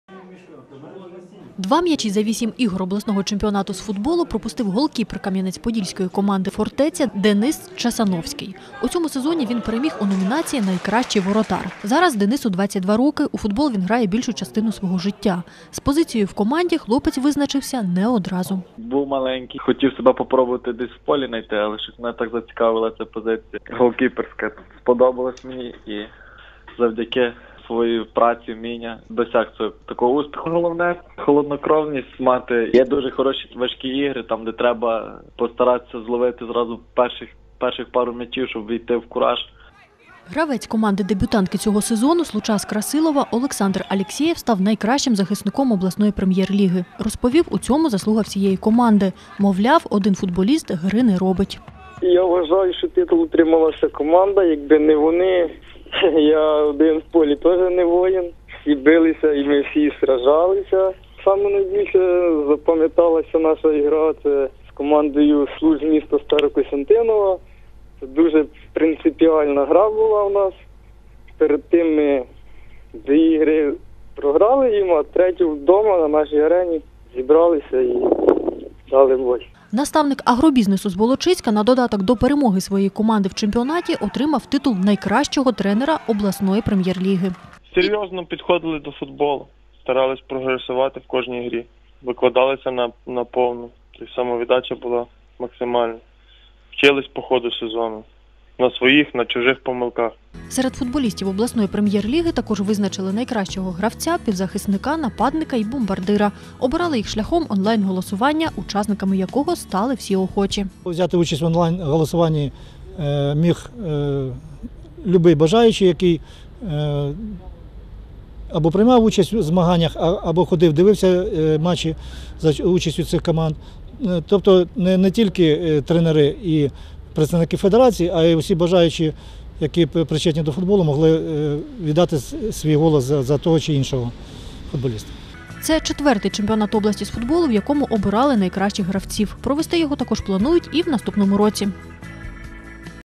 На Хмельниччині обрали найкращих гравців обласної Прем’єр Ліги
На Хмельниччині визначили найкращих гравців обласного чемпіонату з футболу. Онлайн-голосування за них тривало місяць. До нього долучилися більше півтисячі краян. Серед них були і самі футболісти, і ті, кому просто цікавий цей вид спорту. Переможців обирали в семи номінаціях.